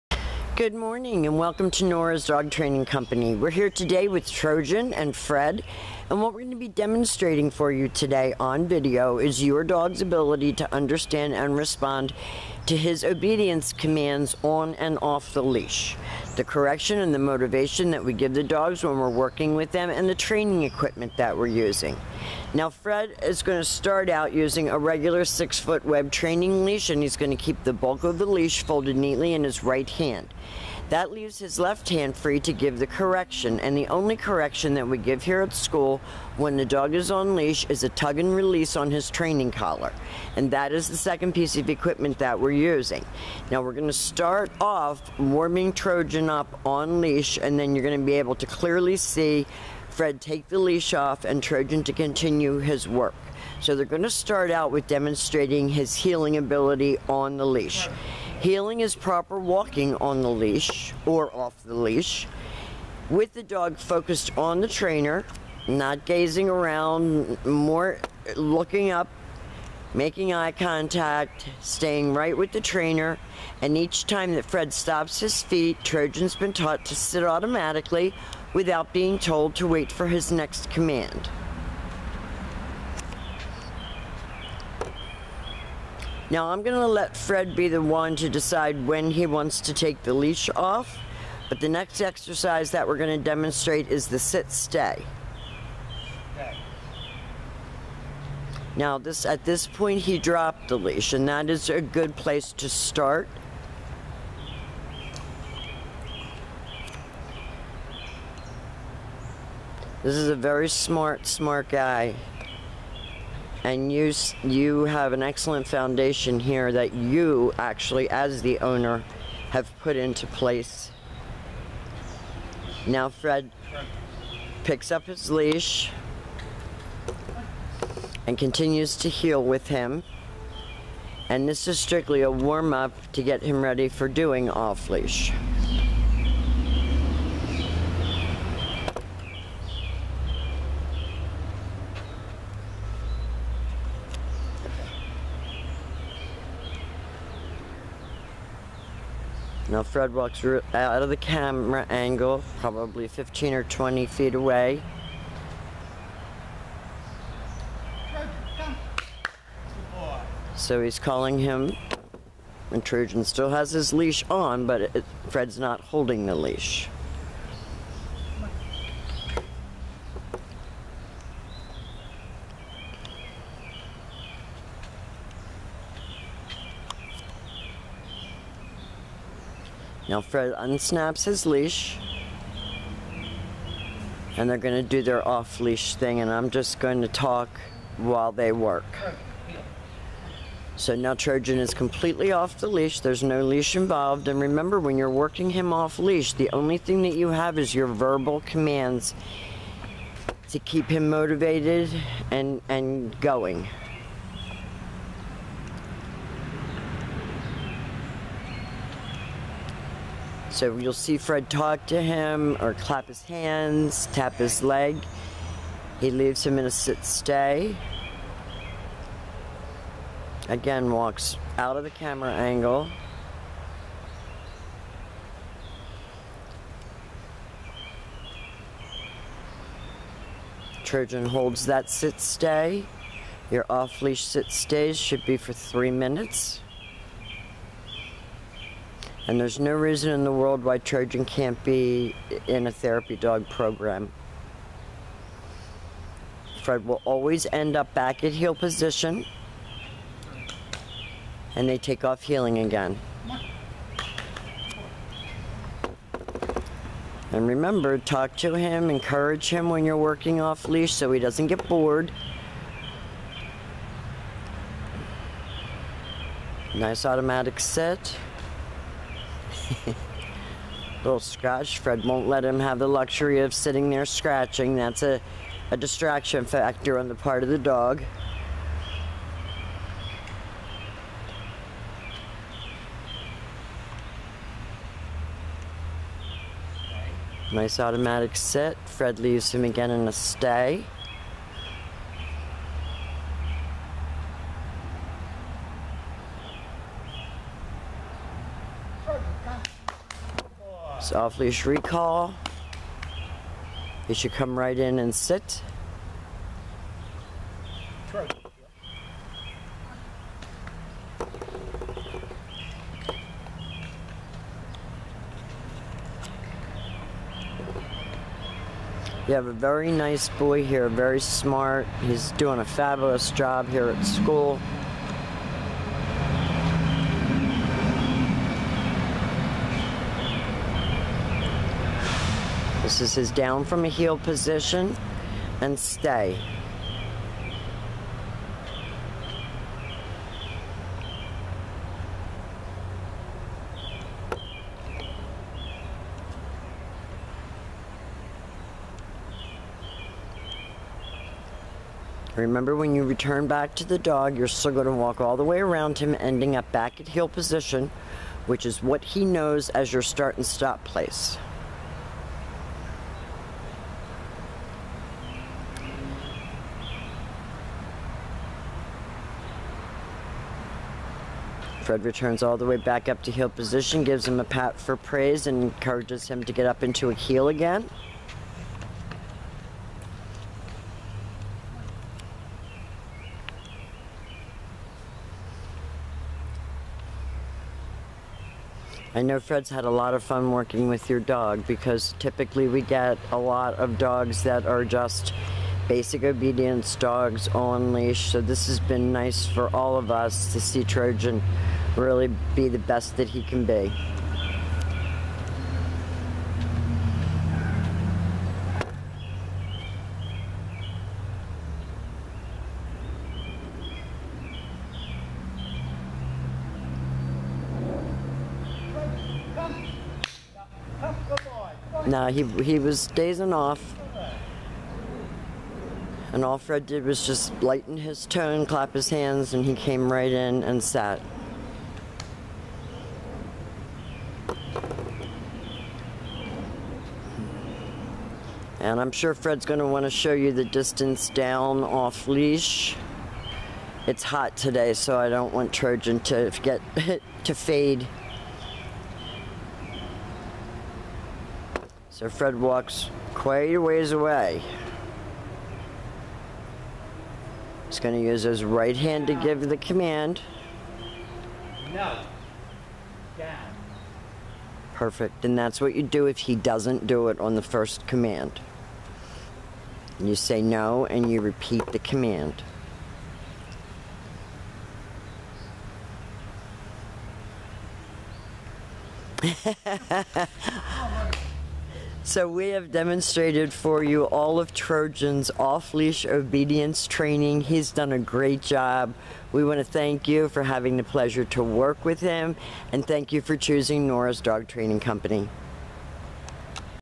Trojan
Trojan the beautiful mature German Shepherd demonstrates off leash obedience commands with his trainer.  He will be moving on to therapy dog training after graduation.